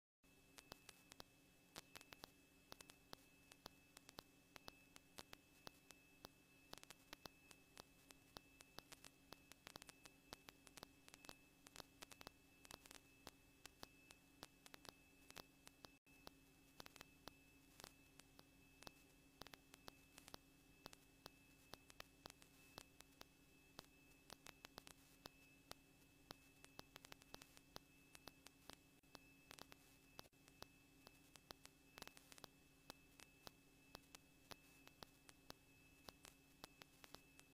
"At least six people have died and dozens injured in Fort Worth pile-
"At least six people have died and dozens injured in Fort Worth pile-up"